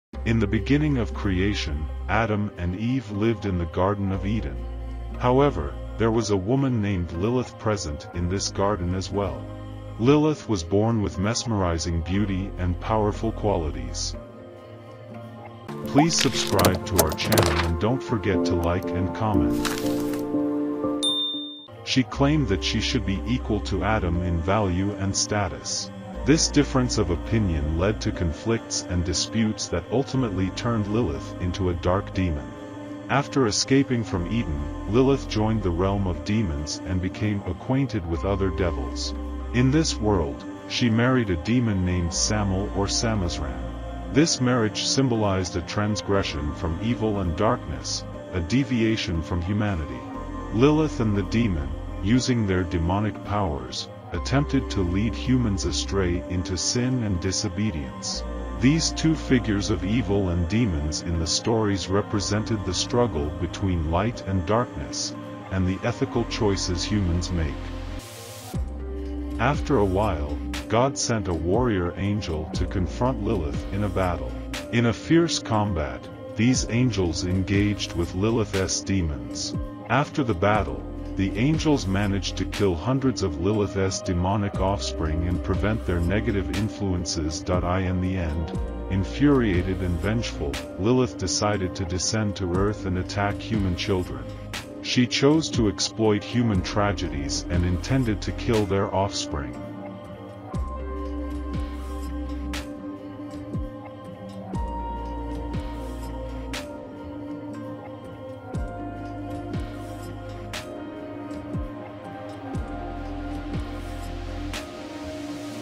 The Tale of Lilith: Struggle Between Light and Darkness
Discover the captivating story of Lilith, a powerful and beautiful being who challenged norms and ultimately turned into a dark demon. From her presence in Eden to her marriage to a demon and the battle with angelic warriors, witness the epic clash between light and darkness, and the ethical choices that define humanity. Explore the symbolism of this ancient myth that continues to resonate through time, portraying the eternal battle between good and evil.